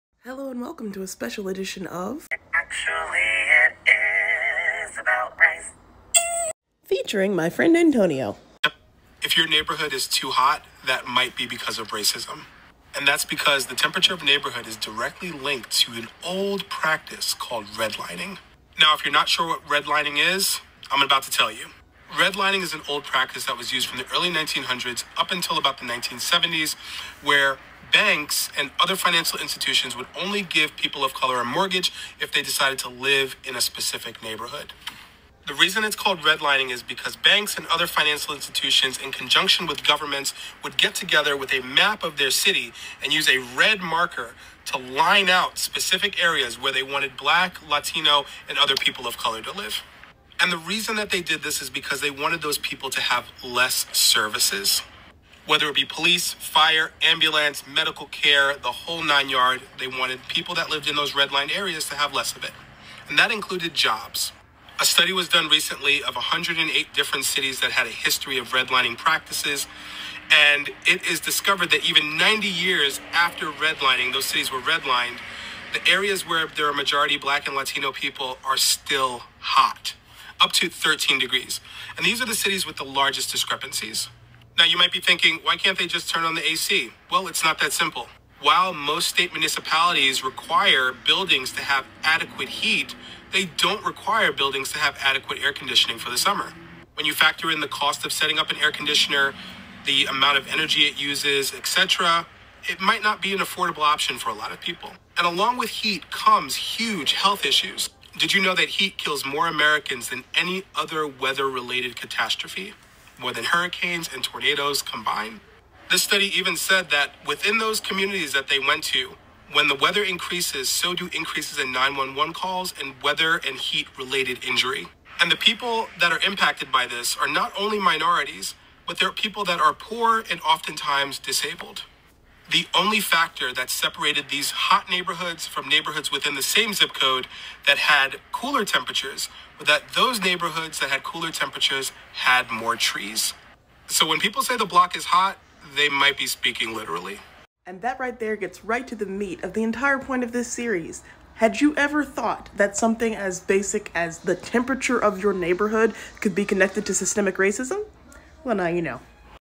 Actually, it is about race! Special episode￼
Did you know the temperature of your neighborhood might be connected to systemic racism?￼